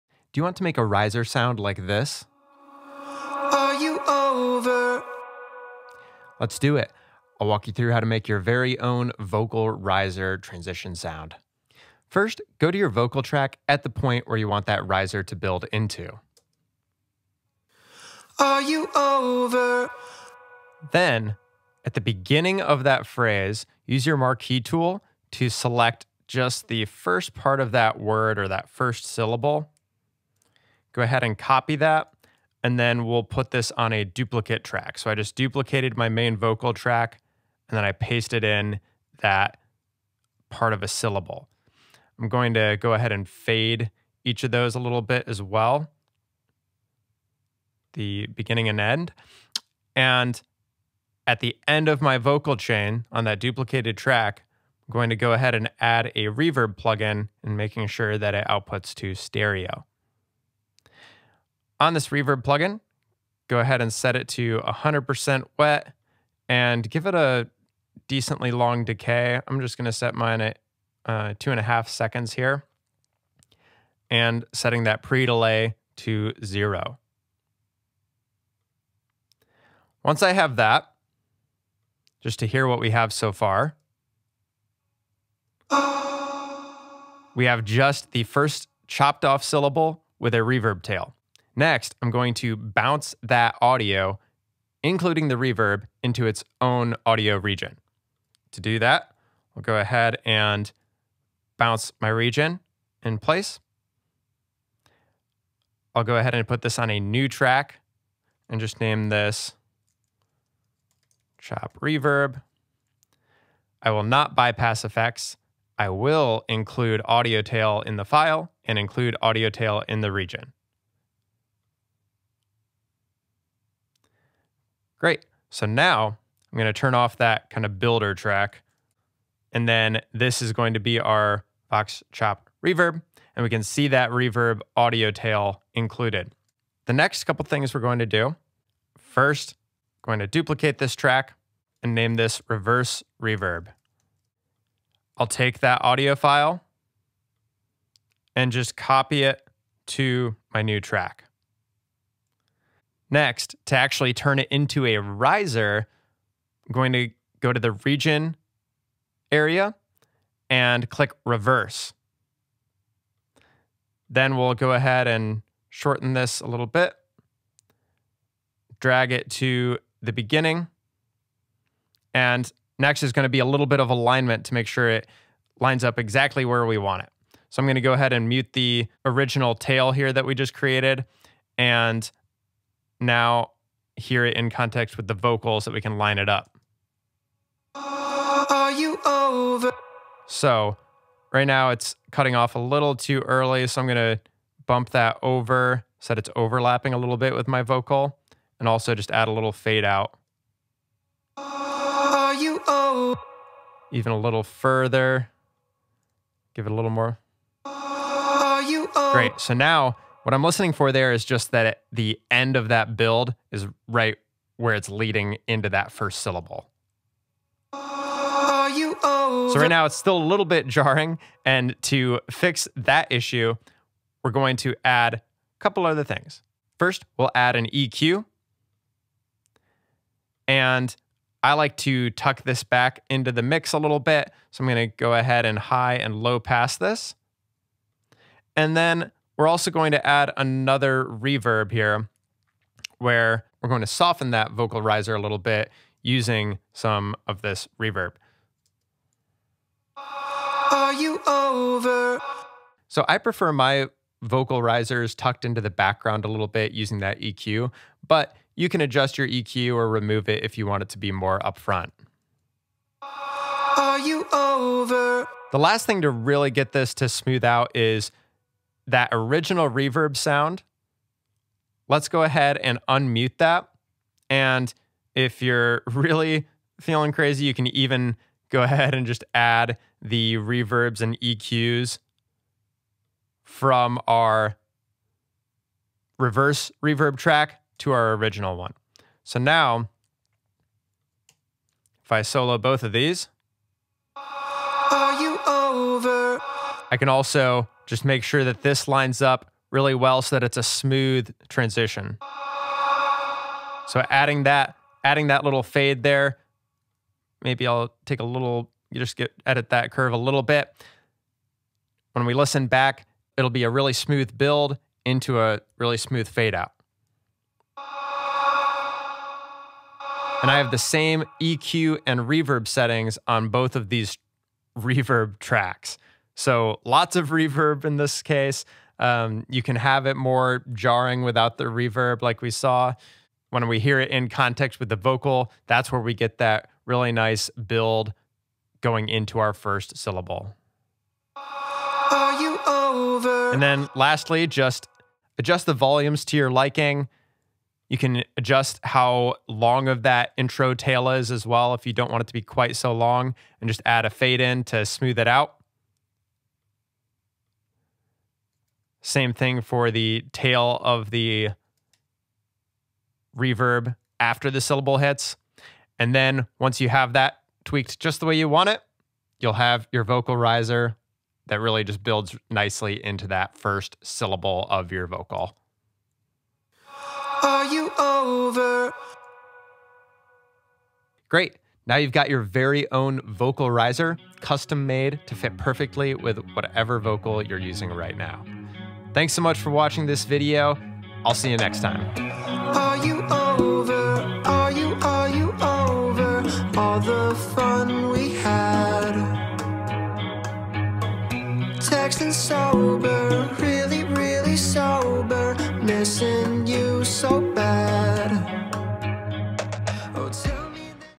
How to Make a Vocal Riser for cool pop song transitions | Cradle Cat | Logic Pro X
How to make a reverse vocal reverb riser! Drop a comment for other videos you'd like to see!

In this video, I walk through how to make that cool vocal riser sound you hear in a bunch of pop songs. It's nice way to add transitions to your song vocals without using additional crash risers, noise risers, or other more percussion based risers. The vocal chop risers are super common in pop songs, so it's a good way make your songs sound more "modern".

Things I'm using in this video:
DAW - Logic Pro X - making a vocal riser transition sound in Logic Pro X
EQ - Logic's stock Channel EQ
Reverb - Logic's stock reverb ChromaVerb